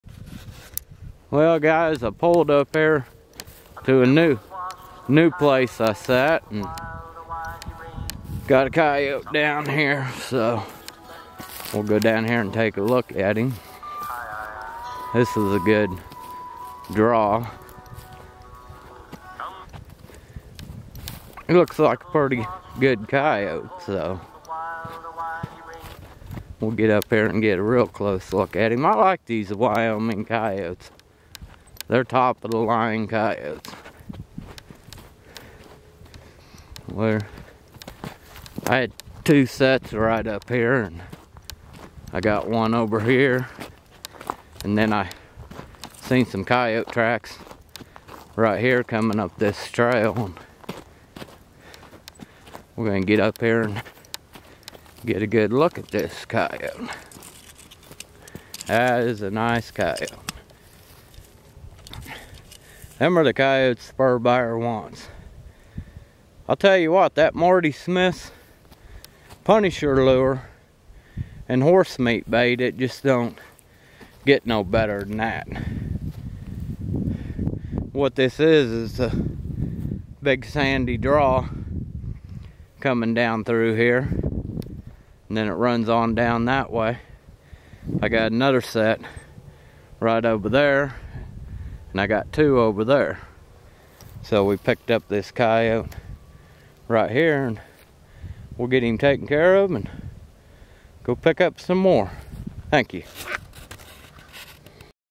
Very Nice Wyoming Coyote
Here is a very nice wyoming coyote caught in a dirt hole st.